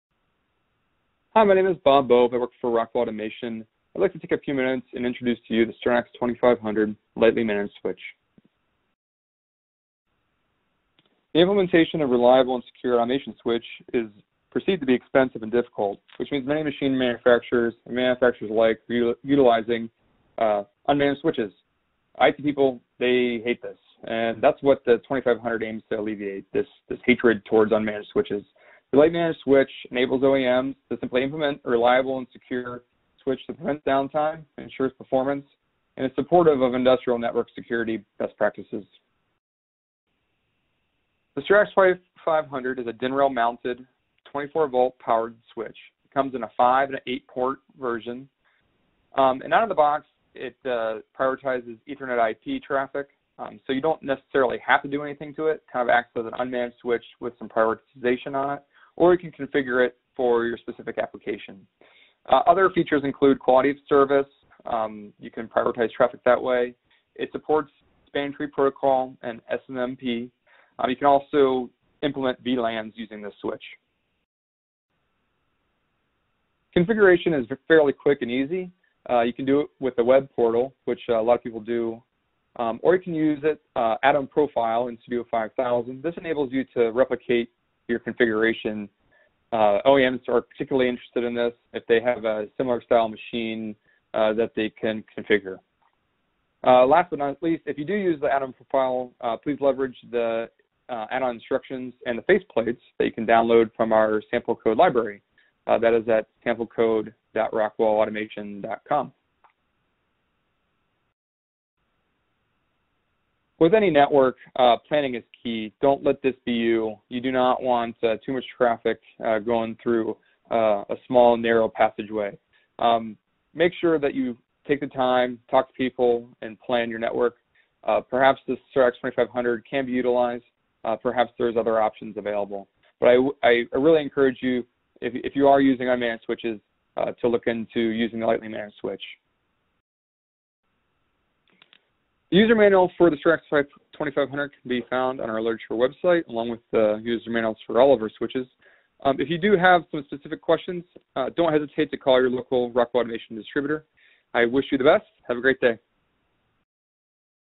Stratix 2500 Networks
Stratix® 2500 Lightly Managed Switches enable network connectivity in applications where traditional unmanaged switches lack the ability to provide diagnostics and security.
This connectivity helps you achieve higher productivity and network reliability.